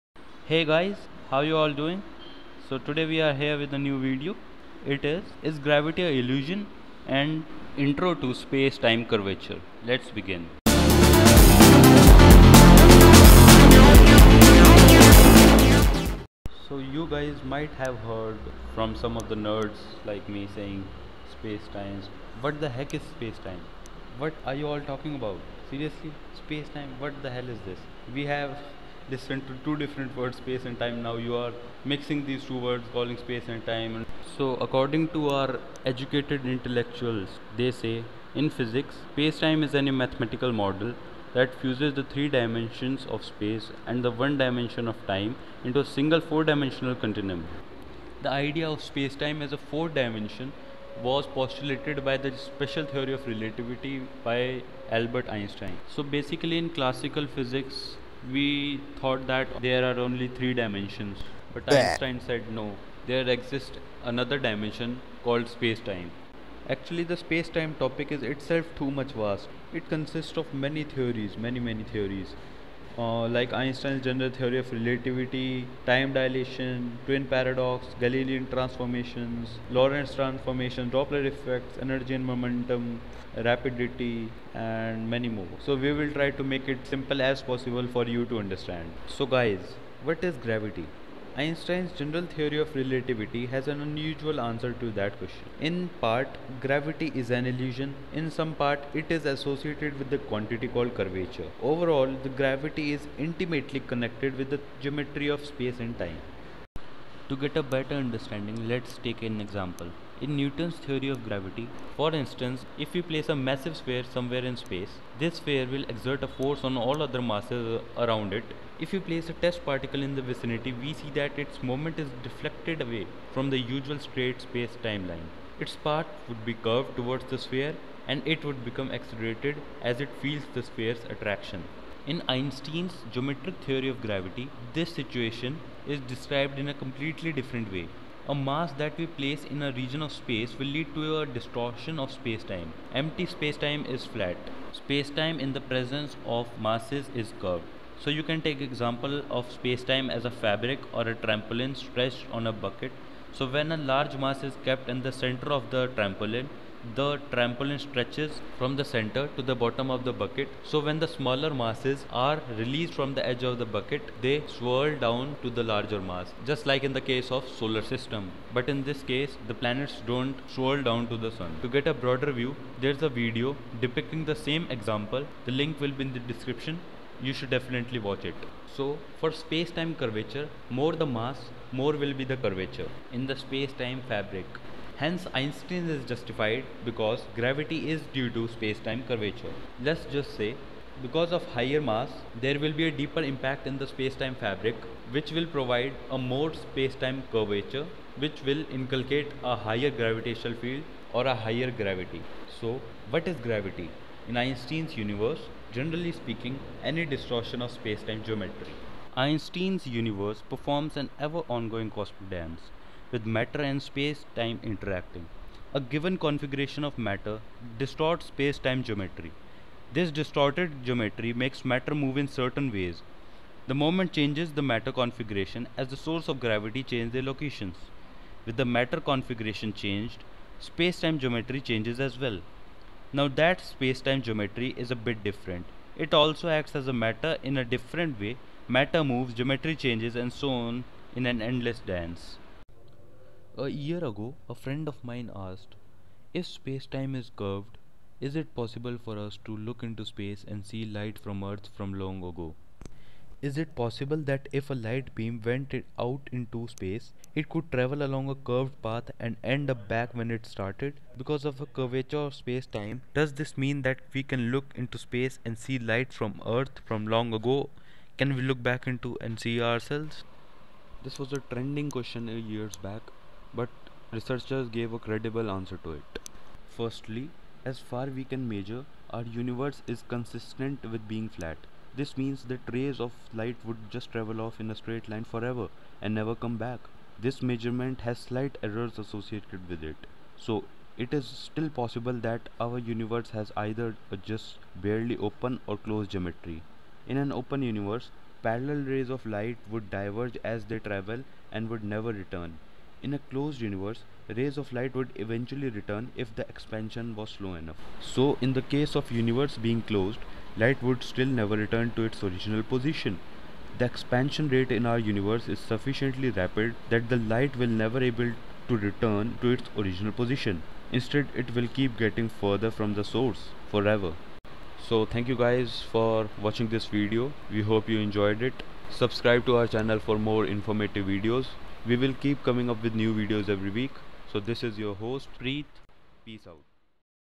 Is Gravity an Illusion? Spacetime Curvature explained !
Is gravity just a term created by humans only ,if it is so then how all of it in space works, what is space time, is it curved, what is the reason behind its curvature, so here's answer to all your queries

Peace out.